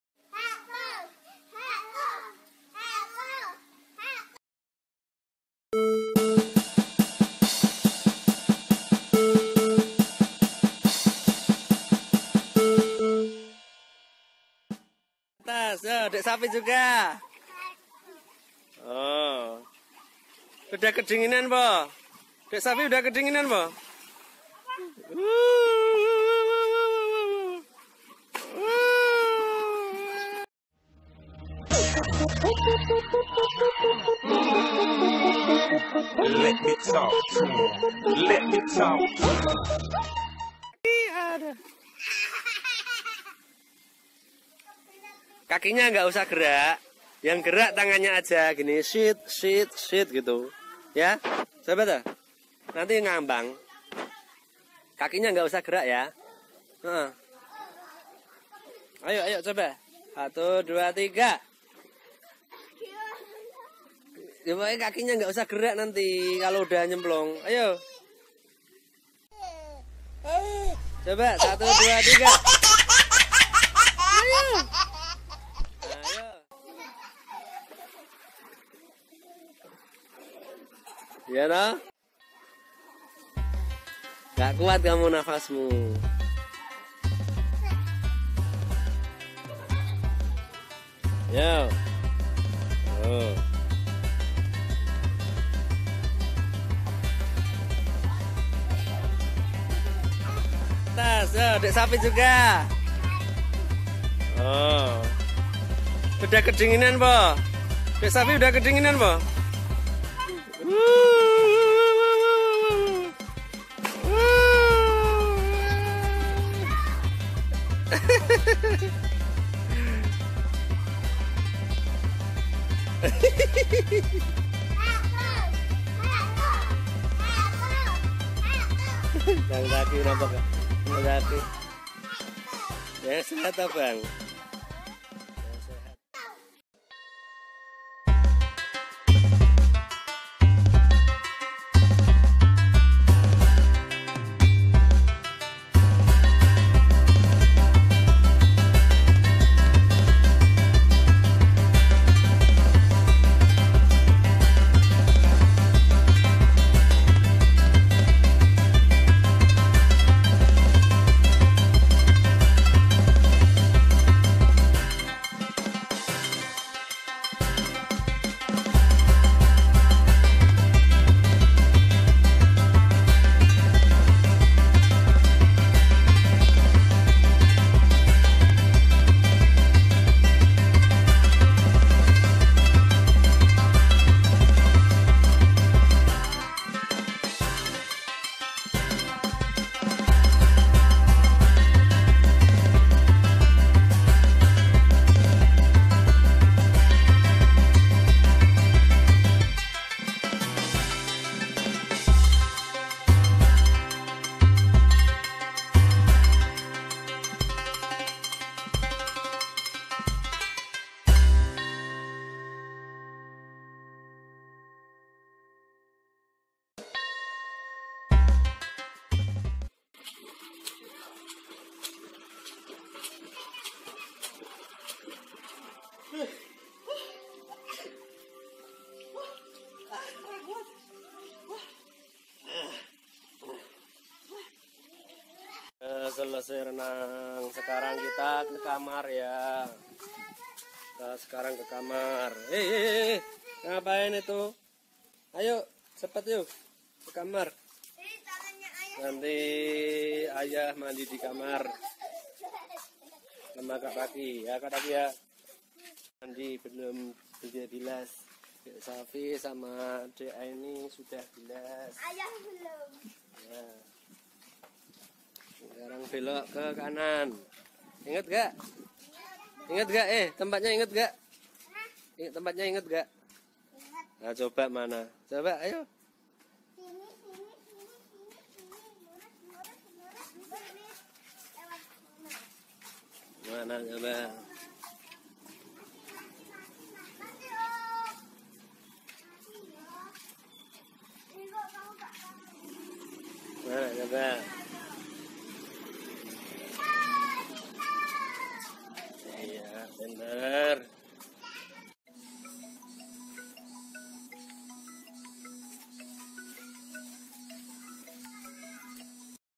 Hostel with swimming poll in Yogyakarta | Kolam Renang Anak Jogja Amazon Green Zone 1
Liburan sekolah diisi dengan hal hal yang bermanfaat ya. Renang juga bagus lo untuk kesehatan selain untuk menghilangkan kebosanan. Selamat menikmati video renang di